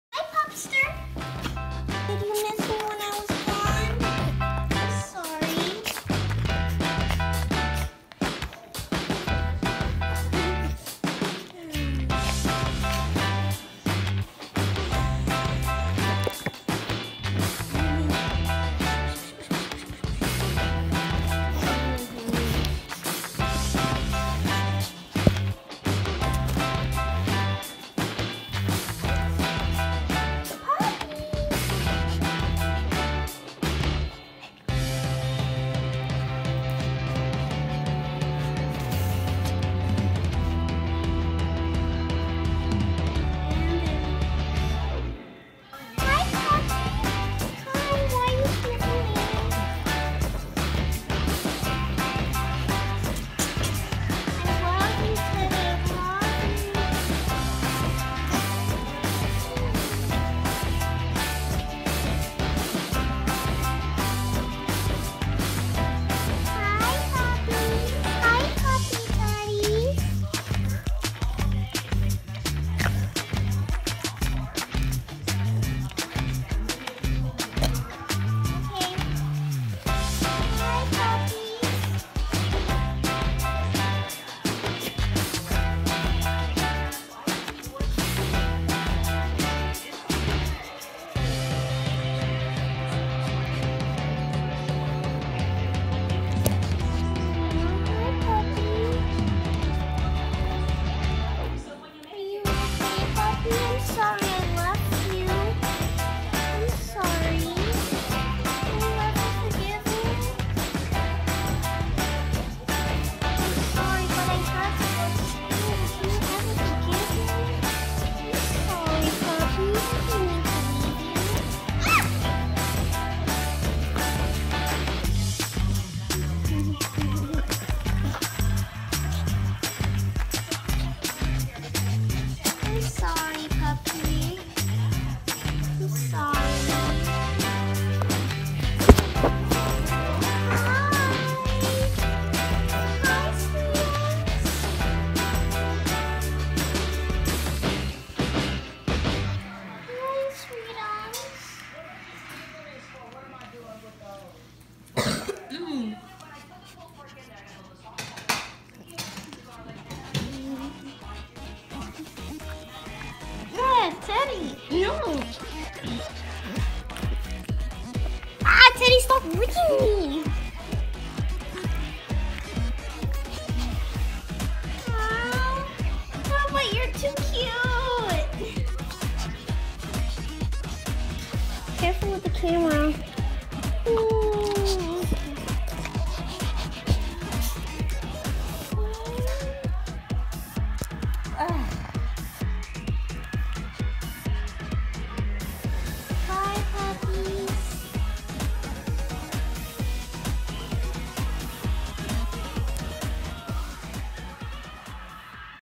Teddy misses me 🐶😭
Teddy missed me today because I was away to school I’m so sorry teddy 😭